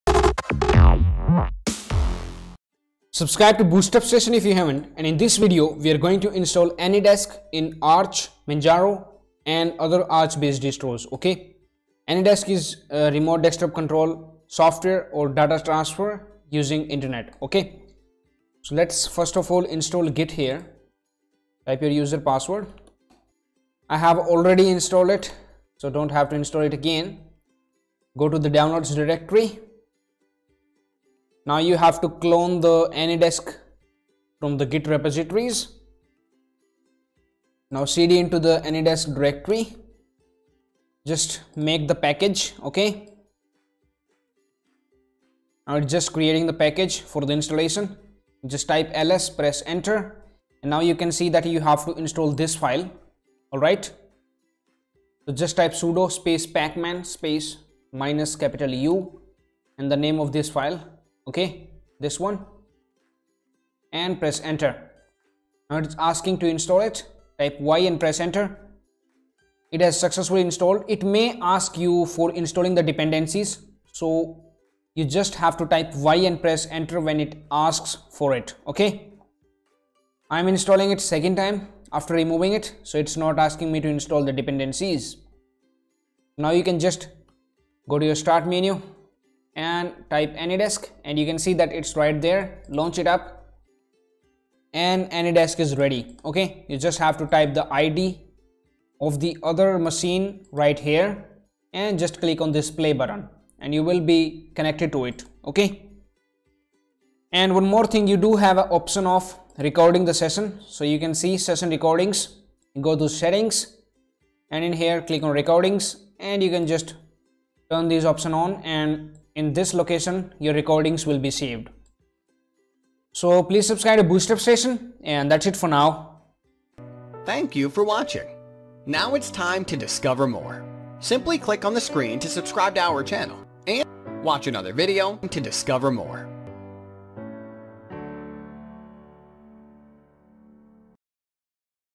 Install AnyDesk in Arch | Install AnyDesk in Manjaro | Fast Remote Desktop Application - AnyDesk
Install AnyDesk in Arch | Install AnyDesk in Manjaro | Fast Remote Desktop Application - AnyDesk in Arch Based Distros with remote control, file transfer, and VPN functionality.